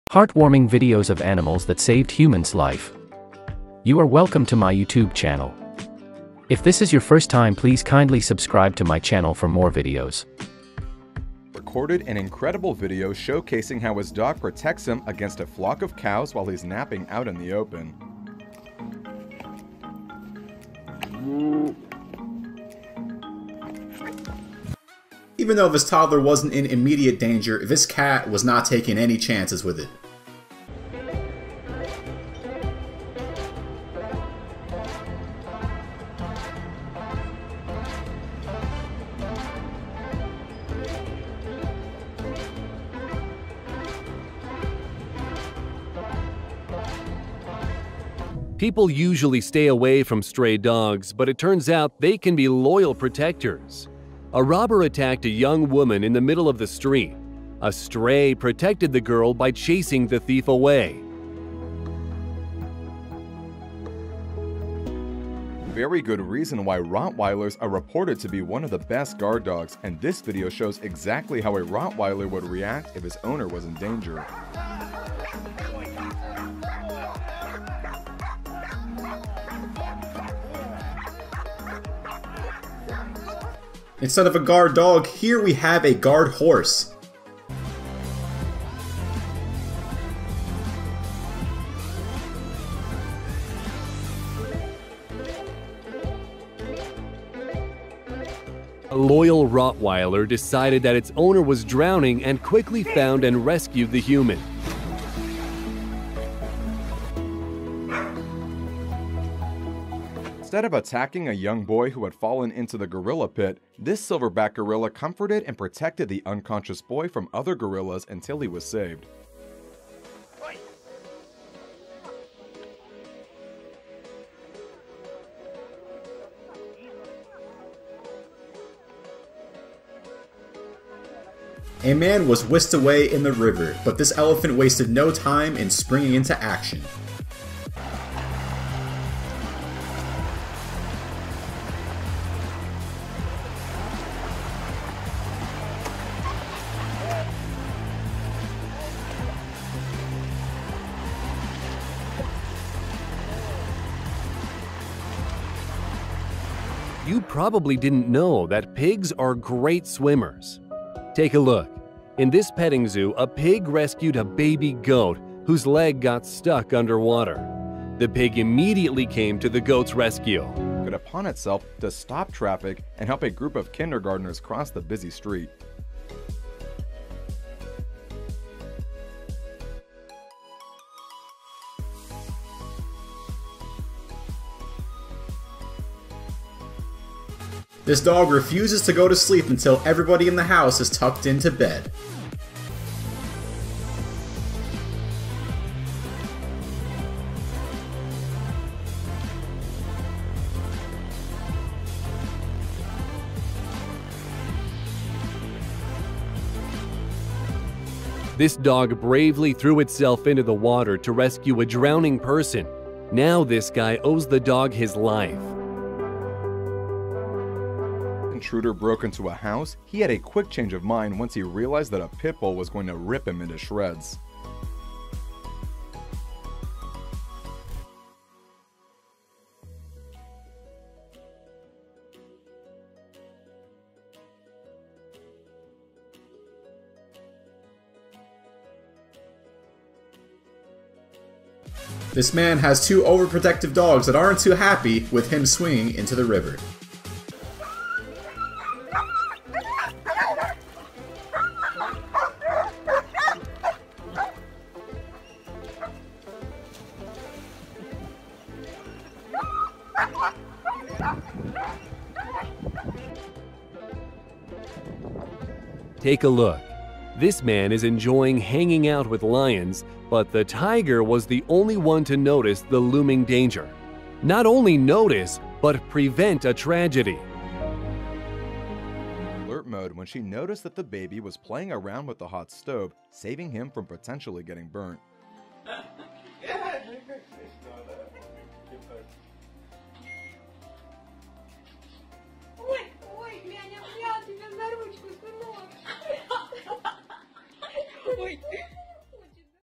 25 Hero Animals That Save And Protect Human Lives !
Here are some heartwarming animals that saved humans lives ! With their reactions you can easily tell how protective they are ! You will not be able to hold your tears.